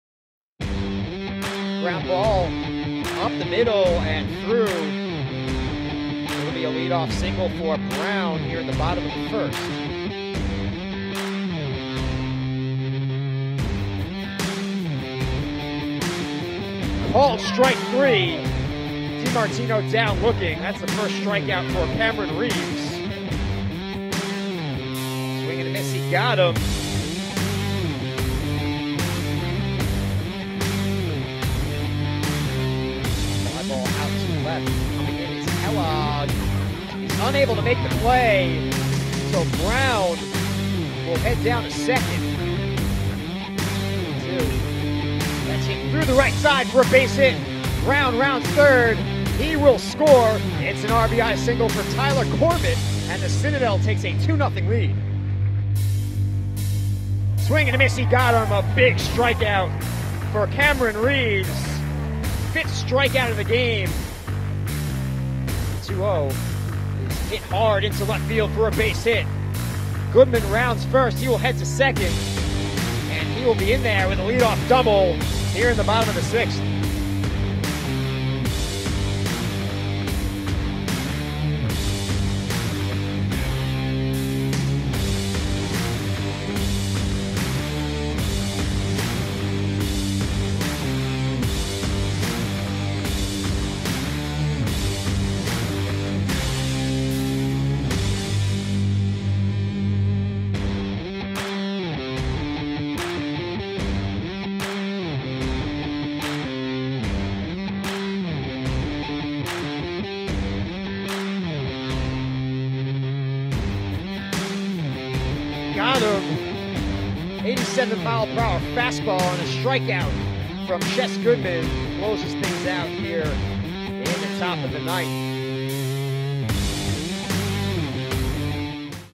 The Citadel Baseball | vs Hartford Game 2
The Citadel defeated Hartford with a final score of 7-0.